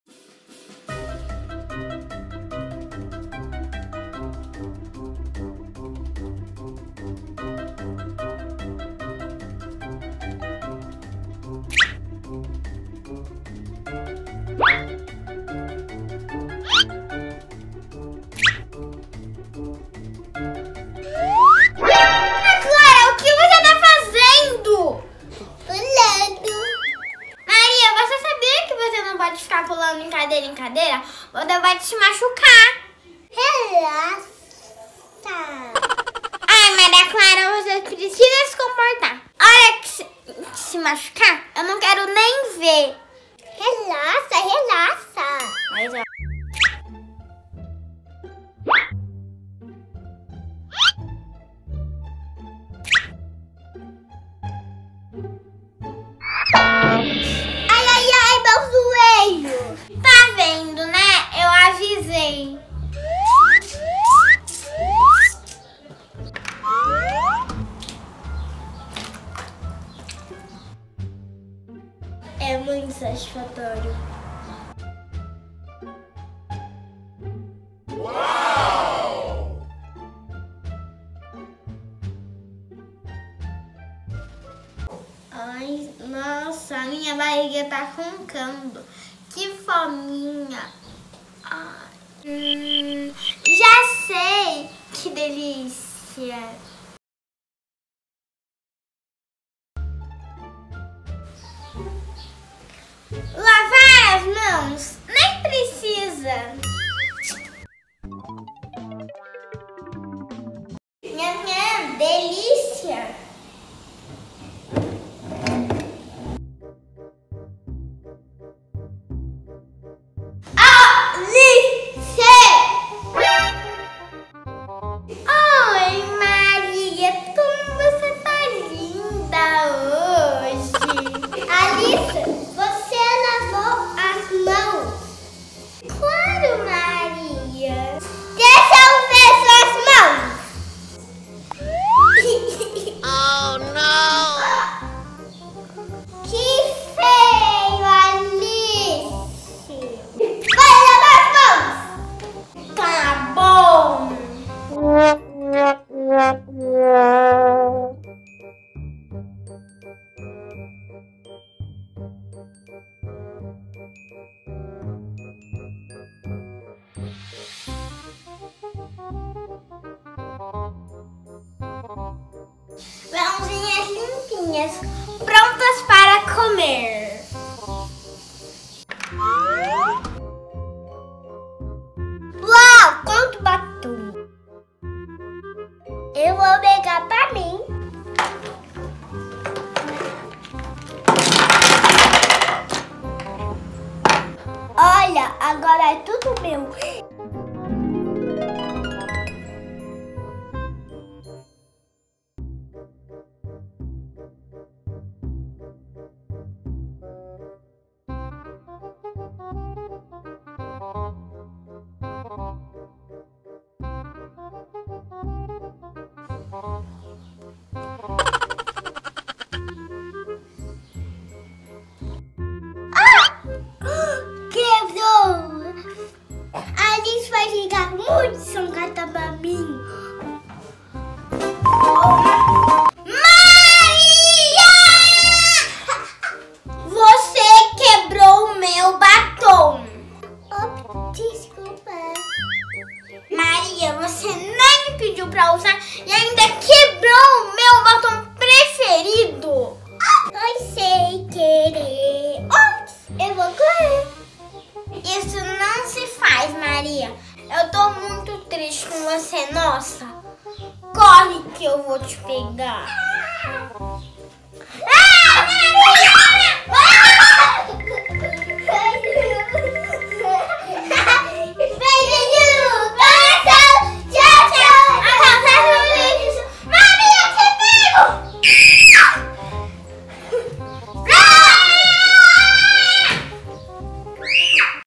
Maria Clara e Alice ensinam Regras de Conduta para Crianças - Rules of conduct for childrens
Rules of conduct for childrens - Maria Clara and Alice
Maria Clara e Alice aprendem e ensinam regras de conduta para crianças como lavar as mãos, pedir emprestado e muito mais!
❤ Facebook:  @Maria Clara e Alice

👉 Observações IMPORTANTES
✔️ Este vídeo possui Legenda em 4 idiomas.